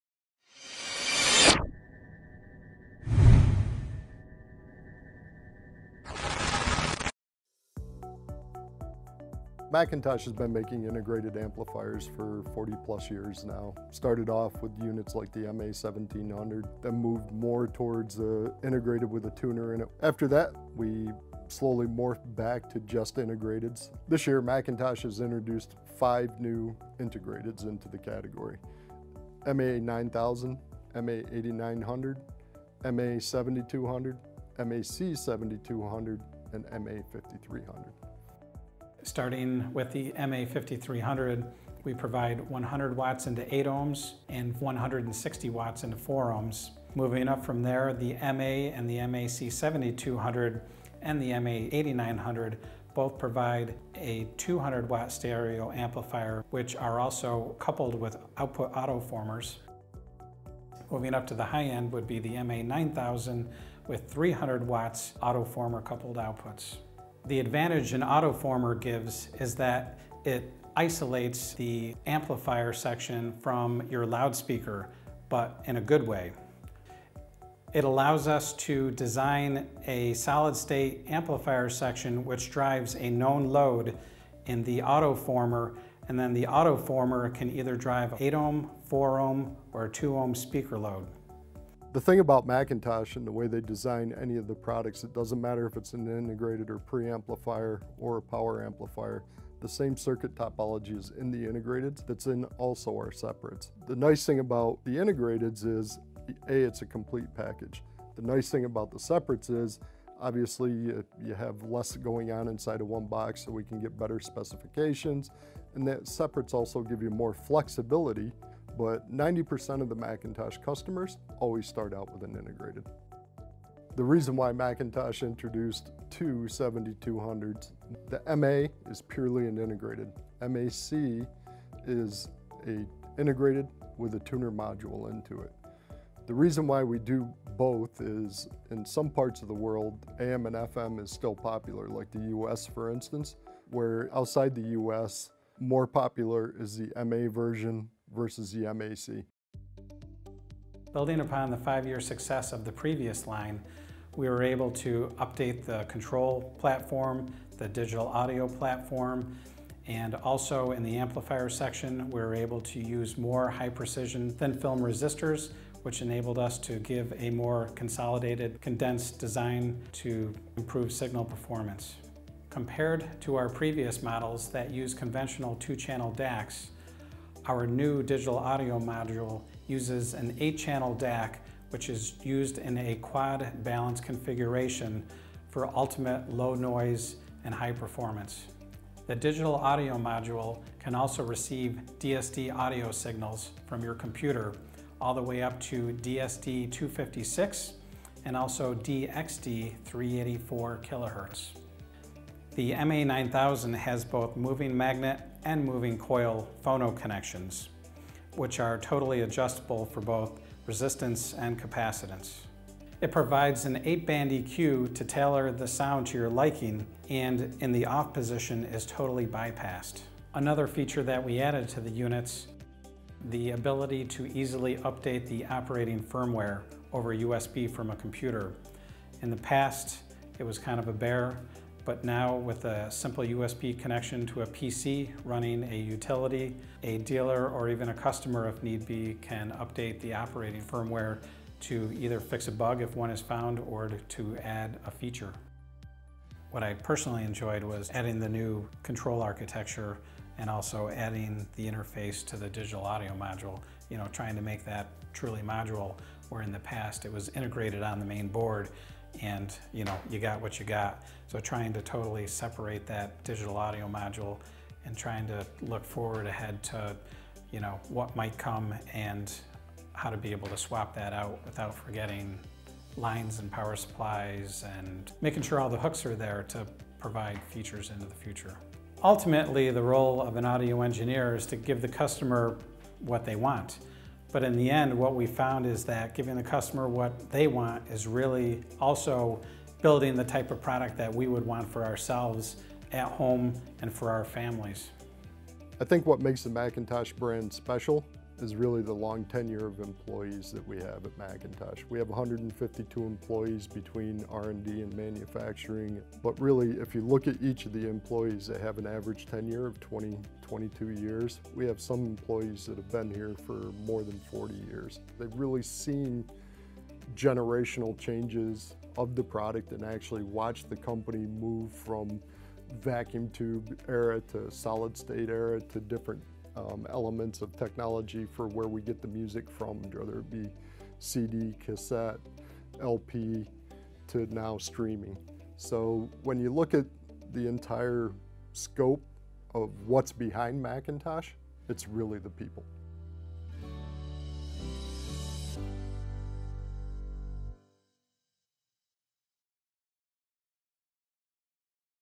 McIntosh's Five New Solid-State Integrated Amplifiers
McIntosh Laboratory's Charlie Randall and John Henkel describe the features of the company's five new solid-state integrated amplifiers: MA9000, MA8900, MA7200, MAC7200, and MA5300. Charlie also talks about the distinct personality that the company has that makes the products made there special. McIntosh Laboratory is located in Binghamton, NY, USA, and has been making audio electronics since 1949.